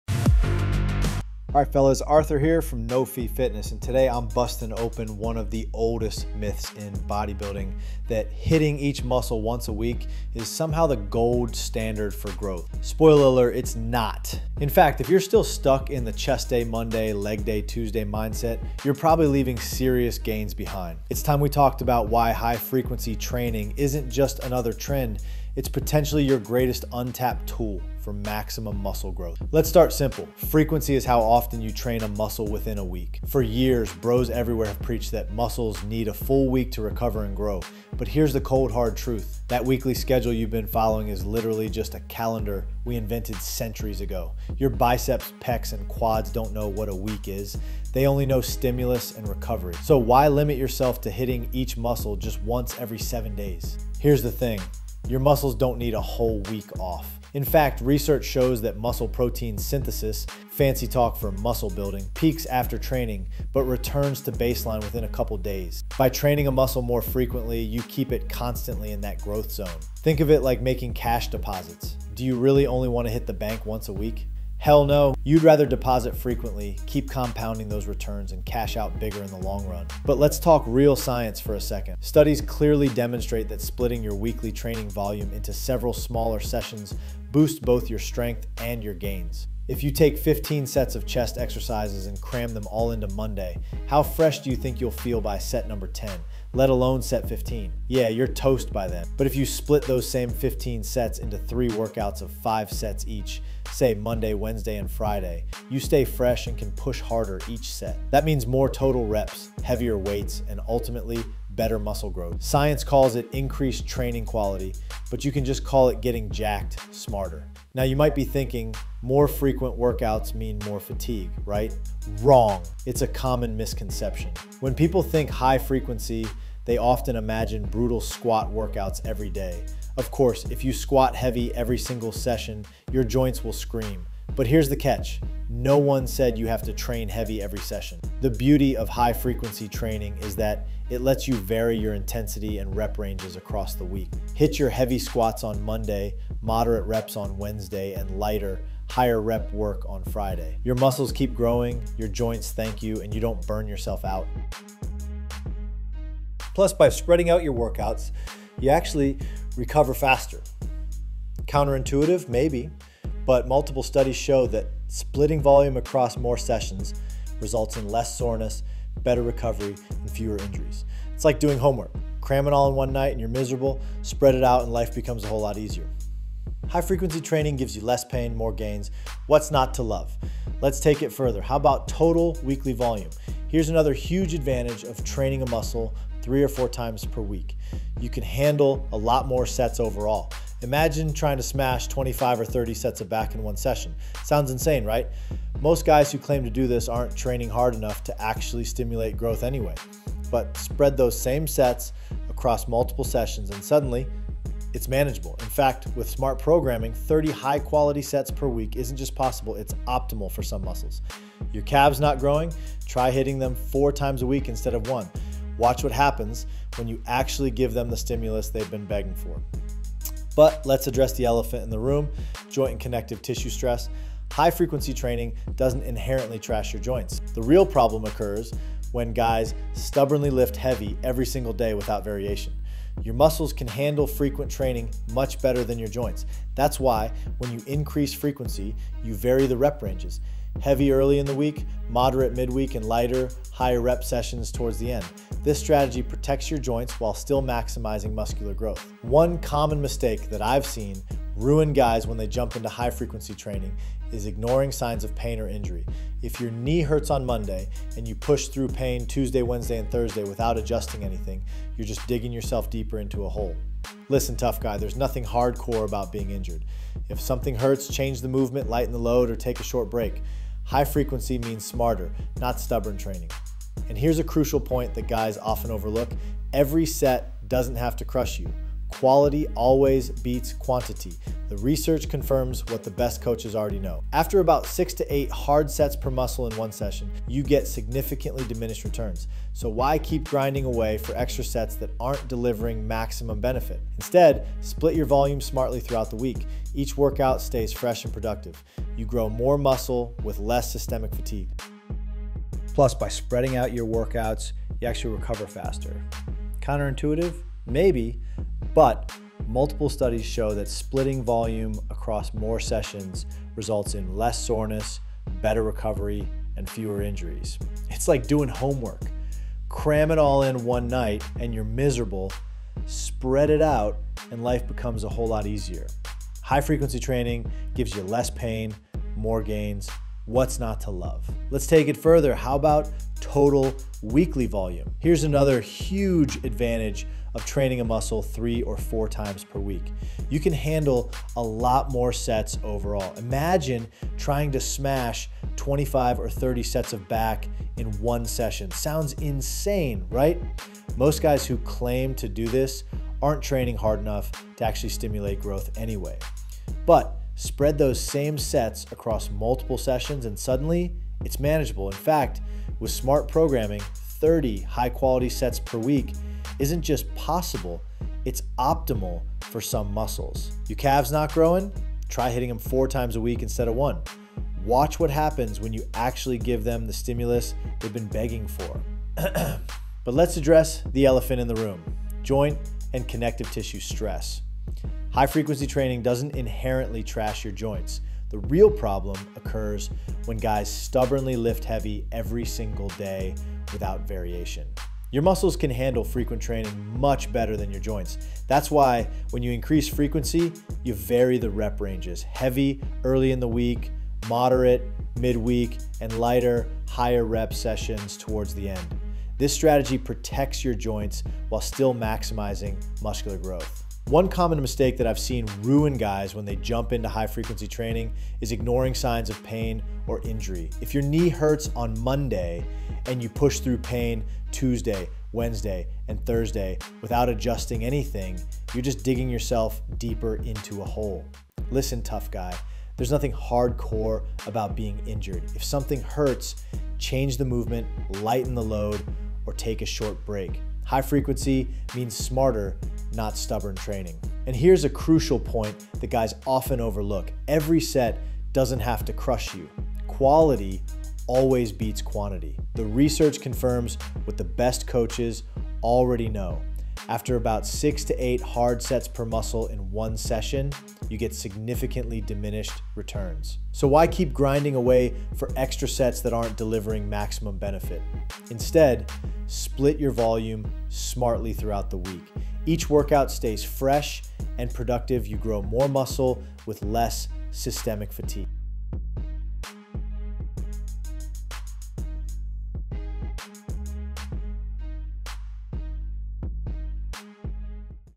Here's Why High Frequency Is the REAL Game Changer
STOP Training Muscles Once a Week—Here's Why High Frequency Is the REAL Game Changer.

Let's start simple—frequency is how often you train a muscle within a week. For years, bros everywhere have preached that muscles need a full week to recover and grow. But here’s the cold, hard truth: that weekly schedule you've been following is literally just a calendar we invented centuries ago. Your biceps, pecs, and quads don’t know what a week is—they only know stimulus and recovery. So why limit yourself to hitting each muscle just once every seven days?
Here's the thing: your muscles don’t need a whole week off. In fact, research shows that muscle protein synthesis—fancy talk for muscle building—peaks after training but returns to baseline within a couple of days. By training a muscle more frequently, you keep it constantly in that growth zone. Think of it like making cash deposits. Do you really only want to hit the bank once a week? Hell no. You’d rather deposit frequently, keep compounding those returns, and cash out bigger in the long run.
But let’s talk real science for a second. Studies clearly demonstrate that splitting your weekly training volume into several smaller sessions boosts both your strength and your gains. If you take 15 sets of chest exercises and cram them all into Monday, how fresh do you think you’ll feel by set number 10, let alone set 15? Yeah, you’re toast by then. But if you split those same 15 sets into three workouts of 5 sets each—say, Monday, Wednesday, and Friday—you stay fresh and can push harder each set. That means more total reps, heavier weights, and ultimately, better muscle growth. Science calls it increased "training quality," but you can just call it getting jacked smarter.
Now, you might be thinking: "More frequent workouts mean more fatigue, right?" Wrong. It's a common misconception. When people think high-frequency, they often imagine brutal squat workouts every day. Of course, if you squat heavy every single session, your joints will scream. But here’s the catch—no one said you have to train heavy every session. The beauty of high-frequency training is that it lets you vary your intensity and rep ranges across the week. Hit your heavy squats on Monday, moderate reps on Wednesday, and lighter, higher-rep work on Friday. Your muscles keep growing, your joints thank you, and you don’t burn yourself out.
Plus, by spreading out your workouts, you actually recover faster. Counterintuitive? Maybe. But multiple studies show that splitting volume across more sessions results in less soreness, better recovery, and fewer injuries. It’s like doing homework—cram it all in one night and you're miserable, spread it out and life becomes a whole lot easier. High-frequency training gives you less pain, more gains—what’s not to love?
Let's take it further—how about total weekly volume? Here's another huge advantage of training a muscle three or four times per week: you can handle a lot more sets overall. Imagine trying to smash 25 or 30 sets of back in one session—sounds insane, right? Most guys who claim to do this aren't training hard enough to actually stimulate growth anyway. But spread those same sets across multiple sessions, and suddenly, it’s manageable. In fact, with smart programming, 30 high-quality sets per week isn’t just possible, it’s optimal for some muscles. Your calves not growing? Try hitting them four times a week instead of one. Watch what happens when you actually give them the stimulus they’ve been begging for.
But let’s address the elephant in the room—joint and connective tissue stress. High-frequency training doesn't inherently trash your joints. The real problem occurs when guys stubbornly lift heavy every single day without variation. Your muscles can handle frequent training much better than your joints. That's why, when you increase frequency, you vary the rep ranges. Heavy early in the week, moderate midweek, and lighter, higher-rep sessions towards the end. This strategy protects your joints while still maximizing muscular growth.
One common mistake that I’ve seen ruin guys when they jump into high-frequency training is ignoring signs of pain or injury. If your knee hurts on Monday, and you push through pain Tuesday, Wednesday, and Thursday without adjusting anything, you're just digging yourself deeper into a hole. Listen, tough guy—there's nothing hardcore about being injured. If something hurts, change the movement, lighten the load, or take a short break. High-frequency means smarter, not stubborn training.
And here’s a crucial point that guys often overlook—every set doesn’t have to crush you. Quality always beats quantity. The research confirms what the best coaches already know: after about six to eight hard sets per muscle in one session, you get significantly diminished returns. So why keep grinding away for extra sets that aren't delivering maximum benefit? Instead, split your volume smartly throughout the week.